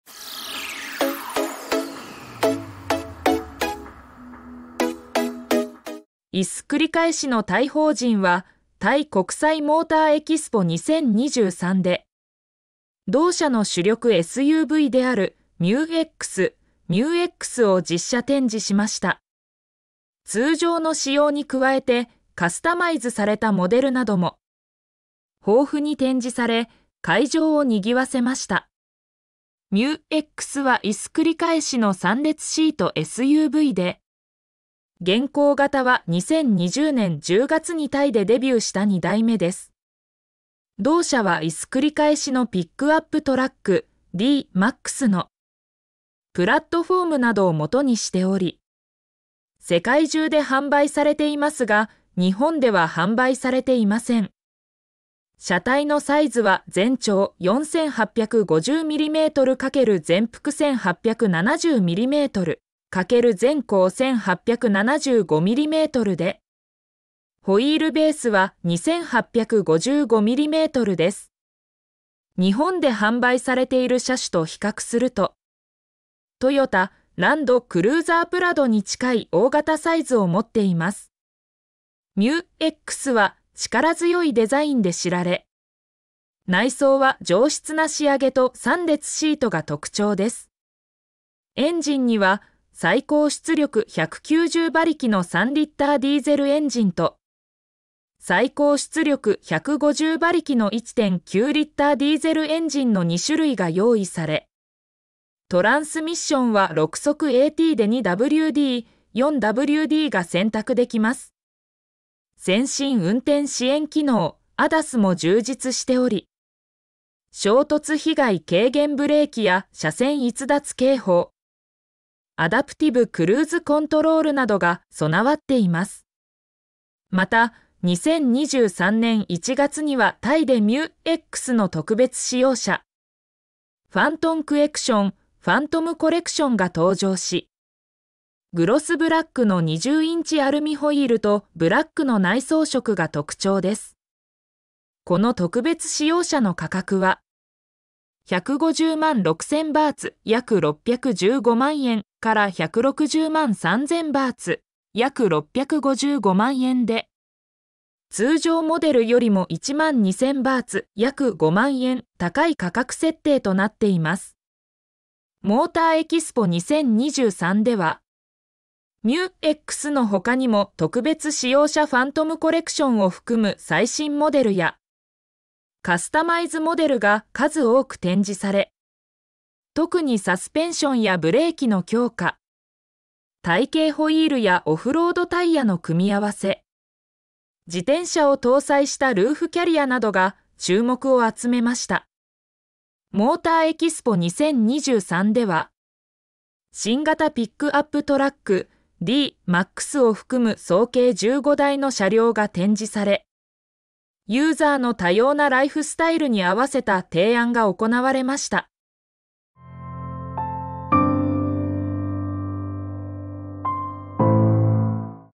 いすゞの「新型MU-X」、タイでデビュー！ | 車の話
MU-Xは、いすゞの3列シートSUV。現行型は2020年10月にタイでデビューした2代目です。

　同社のピックアップトラック「D-MAX」のプラットフォームなどをベースに誕生し、グローバルに展開されていますが日本では販売されていません。